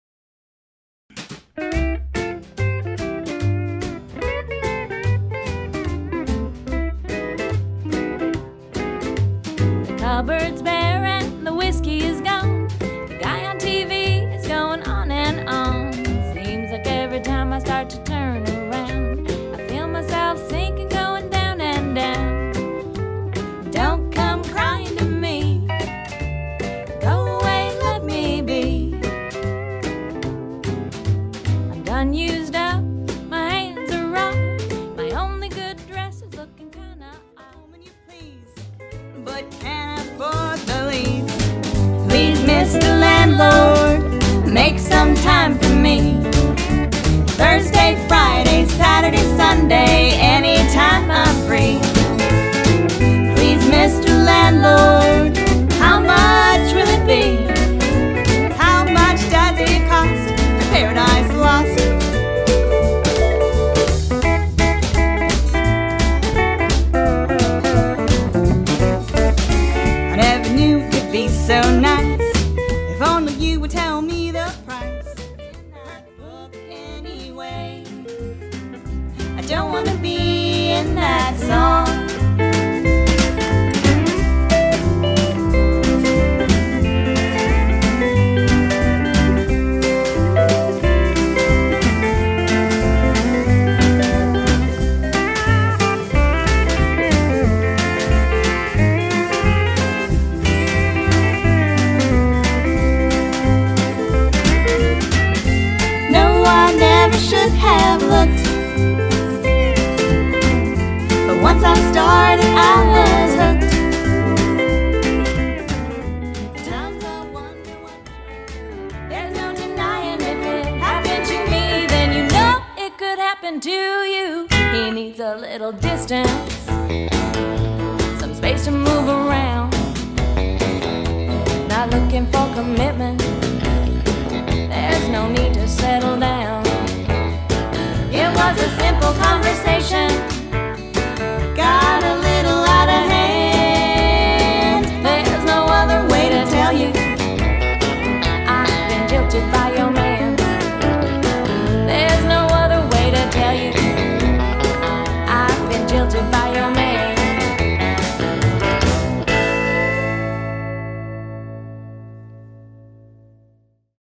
The Better Haves * Debut Album * Due Out early 2016!
The Ranch Party Family Affair known as The Better Haves will be releasing their debut album early in 2016 - Here's a sneak preview of what to expect from this dynamite San Francisco Classic Country Combo!